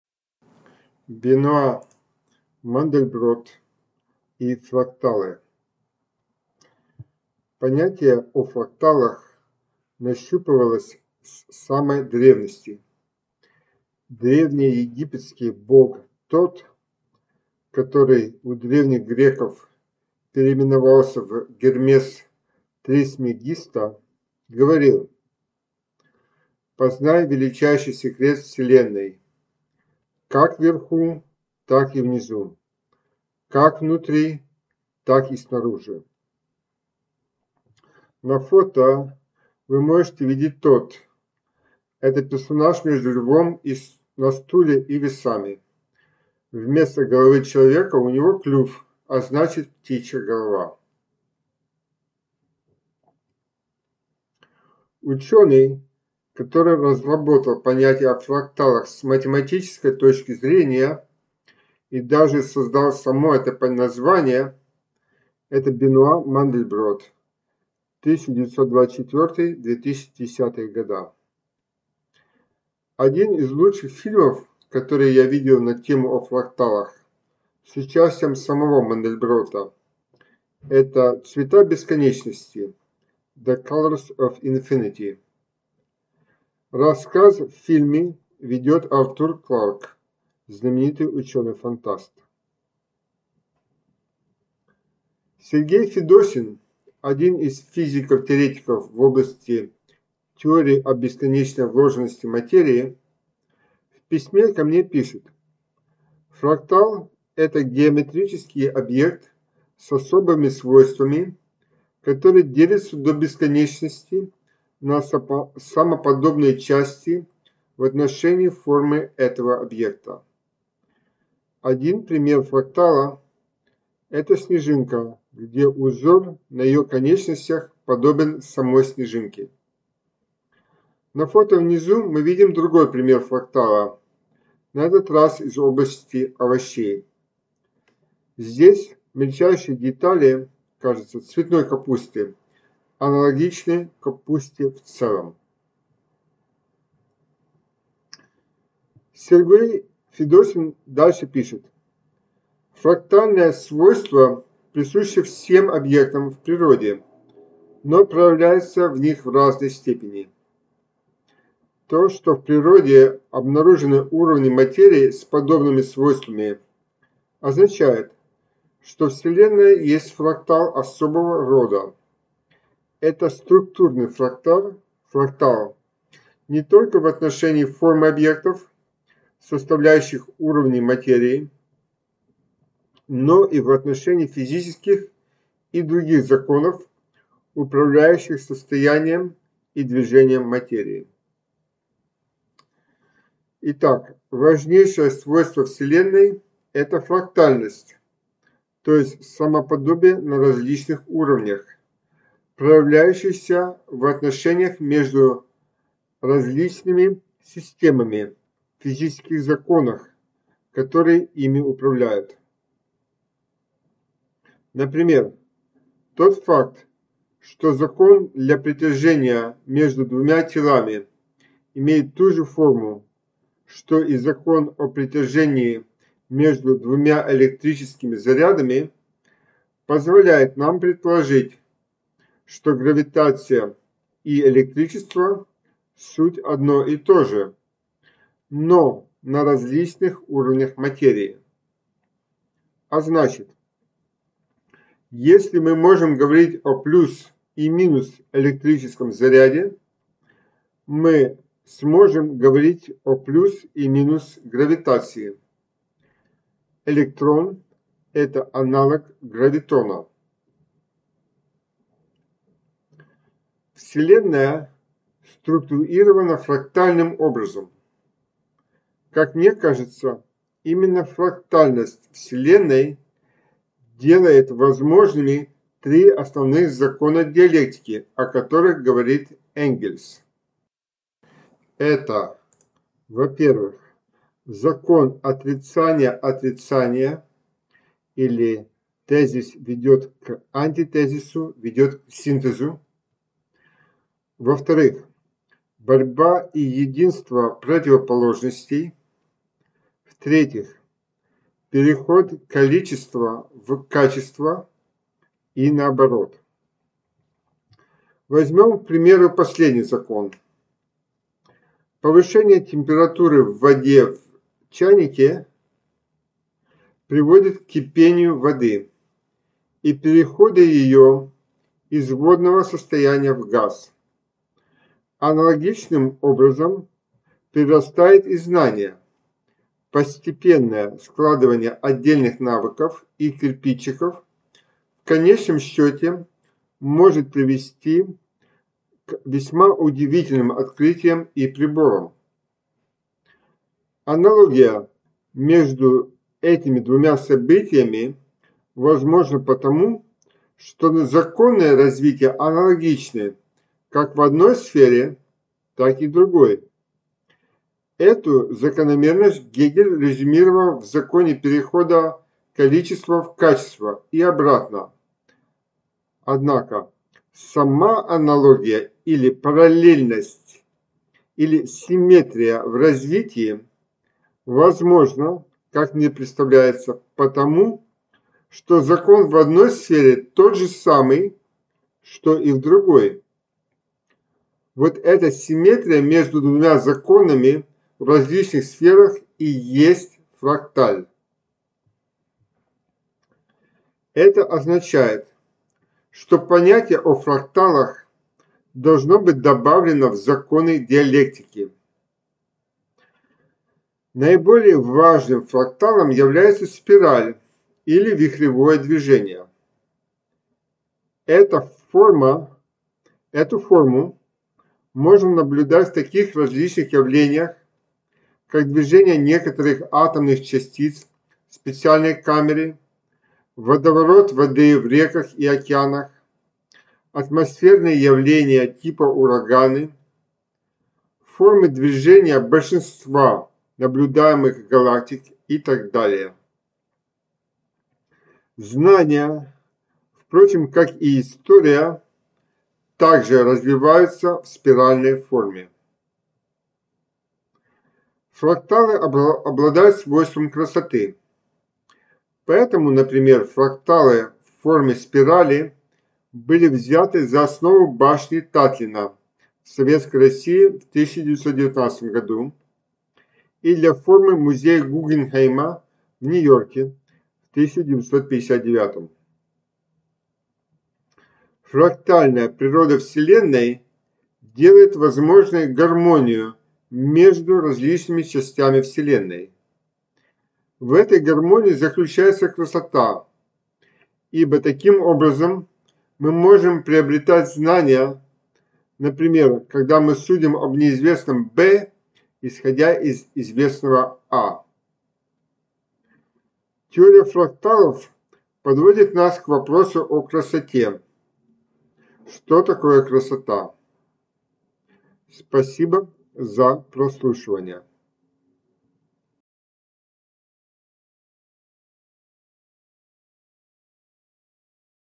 7. Бенуа Мандельбродт и фракталы
Данное видео - часть видео книги по современной диалектике.
В первой части этой книги, мы подходили к диалектике с точки зрения современной философии (т.е. философии после Гегеля). Во второй части, мы рассмативали диалектику с точки зрения логики, математической и другой (например, квантовой). В третьей части, мы подходим к диалектике с точки зрения теории систем. Понятие о фракталах, безусловно, является частью теории систем, ибо система - это некое целое состоящее из частей, а фрактал - это также понятие о соотношении целого и его частей (больше см. видео).